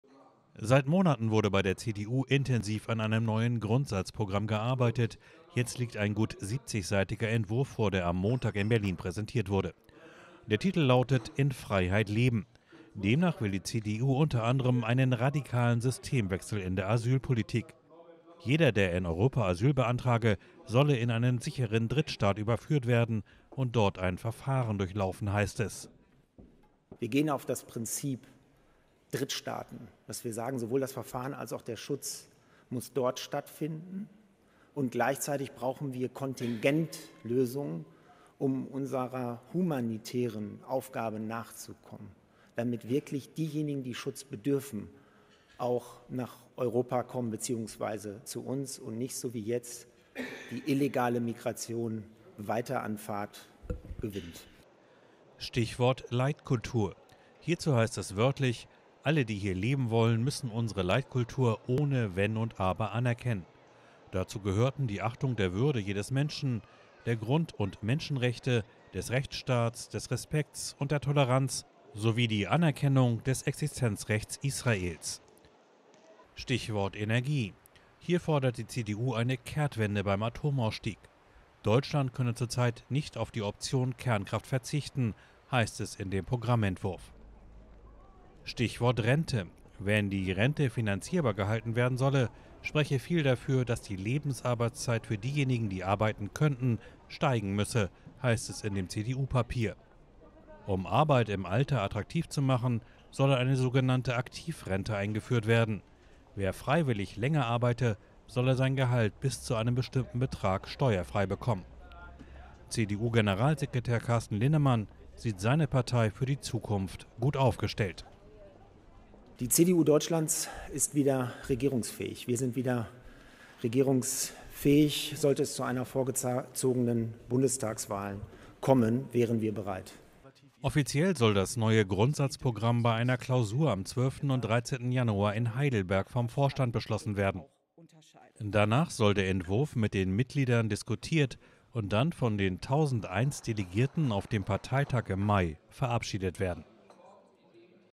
CDU will Asylanträge für EU-Länder in Drittstaaten prüfen lassen
In ihrem Entwurf für ein neues Grundsatzprogramm setzt die CDU auf einen radikalen Systemwechsel in der Asyl- und Migrationspolitik. Jeder, der in Europa Asyl beantrage, solle in einen sicheren Drittstaat überführt werden, heißt es in dem Papier.

„Die CDU Deutschlands ist wieder regierungsfähig“, erklärte der Leiter der Programmkommission, Parteivize und Generalsekretär Carsten Linnemann. Die CDU habe ihr „Wertefundament erneuert und bekräftigt“, sagte er bei der Vorstellung am Montag.